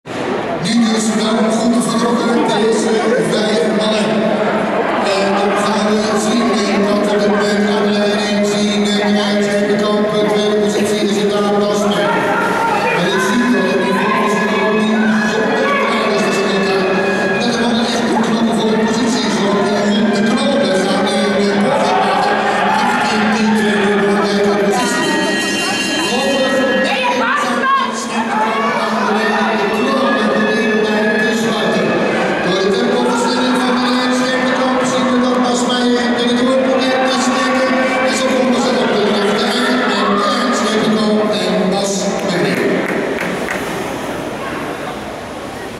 NK 1/4 finale 500m Jun B H2
Dutch Championship 3-6-2017, Heerenveen
M Scheperkamp, B Mijer, J de Kroon, N Stolhof, T Kalthof